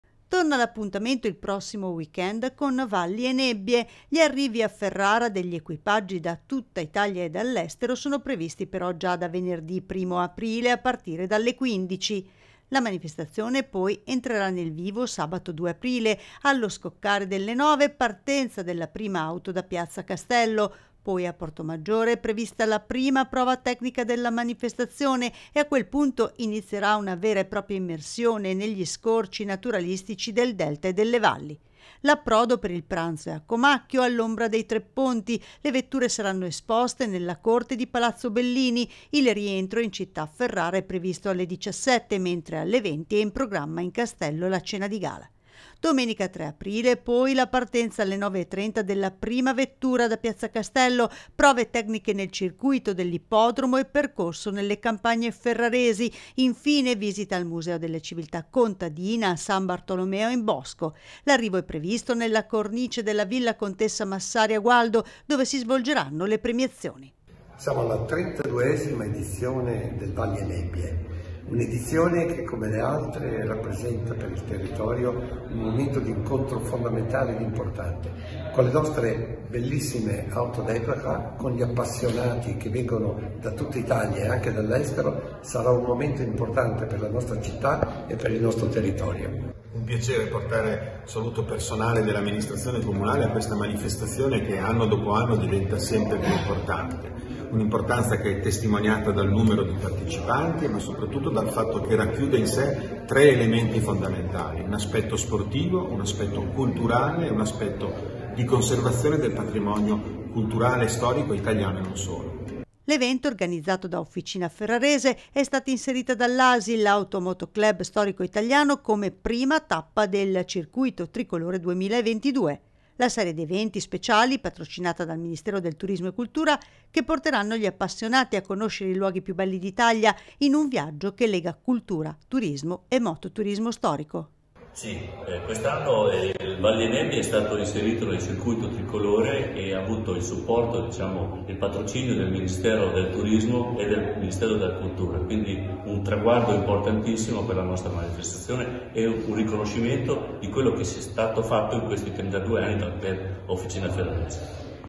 "Valli e Nebbie" torna dall'1 al 3 aprile con la 32esima edizione
"Un ritorno alla grande, dopo anni difficili, con ancor più voglia di gustare gli scorci meravigliosi del nostro territorio a bordo dei bolidi che hanno scritto la storia dell'automobilismo".

Riccardo Zavatti, presidente del Club Officina Ferrarese del Motorismo Storico è impaziente. Non vede l'ora che arrivi il primo fine settimana di aprile per tagliare il nastro della 32esima edizione del 'Valli e Nebbie'.

"E' un traguardo importante - così il presidente del Club - e, quest'anno più che mai, la nostra rassegna si conferma piuttosto attesa tra gli appassionati delle quattro ruote storiche ma non solo". In effetti, l’obiettivo massimo per le iscrizioni alla manifestazione è stato ampiamente raggiungo e superato da qualche tempo. Alla griglia di partenza, si presenteranno 75 equipaggi. Apripista sarà Alessio De Angelis (sponsor) a bordo della mitica Bugatti 37 in coppia con il presidente nazionale ASI (Automoto Club Storico Italiano), Alberto Scuro.

l programma: venerdì 1 aprile, sono previsti gli arrivi degli equipaggi da tutt’Italia e dall’estero,in piazza Castello a partire dalle 15. Poi, dalle 16,30, il club ha organizzato per gli equipaggi un tour guidato alla scoperta della città medievale. La manifestazione entra nel vivo sabato 2. Allo scoccare delle 9, partenza della prima auto da Piazza Castello. A Portomaggiore è prevista la prima prova tecnica della manifestazione. A quel punto inizierà la vera e propria immersione negli scorci naturalistici del nostro Delta e delle Valli. L'approdo, per il pranzo, è a Comacchio, all’ombra dei Trepponti. Le vetture saranno esposte nella corte di Palazzo Bellini. Il rientro in città, tradizionalmente accolto da un bagno di folla, è previsto alle 17, mentre alle 20 è in programma, in Castello, la cena di gala. All'indomani, partenza alle 9,30 della prima vettura, sempre da piazza Castello. Prove tecniche nel circuito dell'Ippodromo e percorso nelle campagne ferraresi. Infine, visita al museo della Civiltà contadina a San Bartolomeo in Bosco. L’arrivo è previsto nella cornice della Villa Contessa Massari a Gualdo. Dove si svolgeranno le premiazioni. "E' un programma particolarmente ricco - incalza il presidente Zavatti - e, ci auguriamo, che possa soddisfare le alte aspettative coltivate dai tanti partecipanti". Il Valli e nebbie si conferma sempre più un evento di qualità. Tanto che Asi (Automotoclub storico italiano), ha inserito l’evento organizzata da Officina Ferrarese come prima tappa del 'Circuito tricolore 2022',  la serie di eventi speciali (patrocinata dai ministeri di Turismo e Cultura) che porteranno gli appassionati a conoscere i luoghi più belli d’Italia, in un viaggio che lega cultura, turismo e motorismo storico.